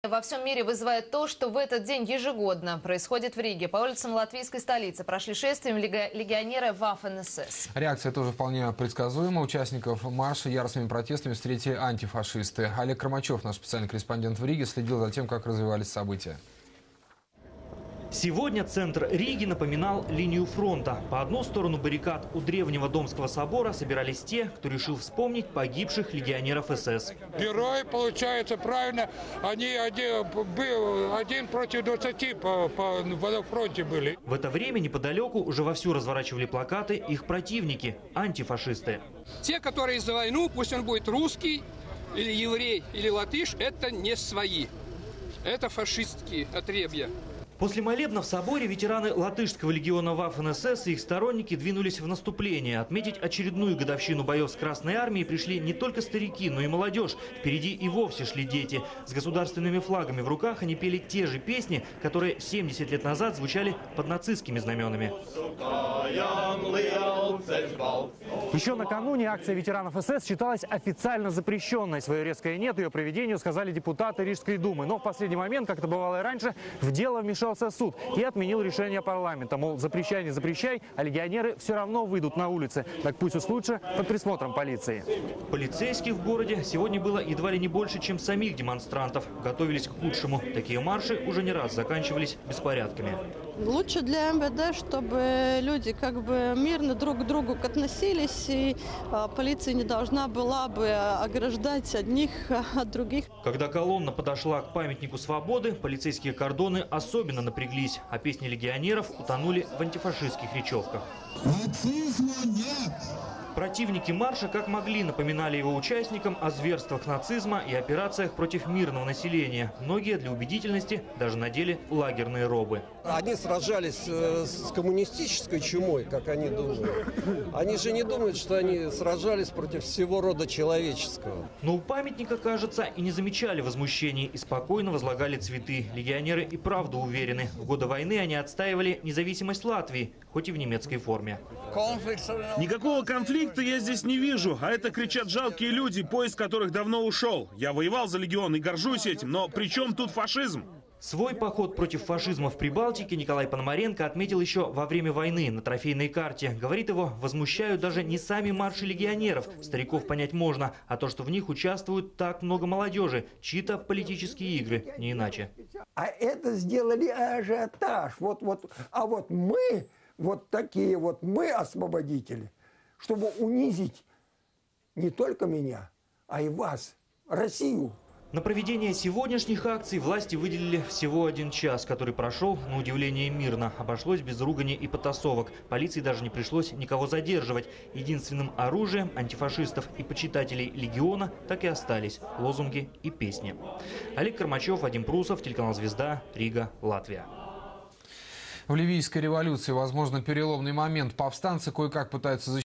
Марш легионеров латышской дивизии "Ваффен СС" в Риге
Очередной марш легионеров латышской дивизии "Ваффен СС"прошел  в Риге 16 марта 2011 года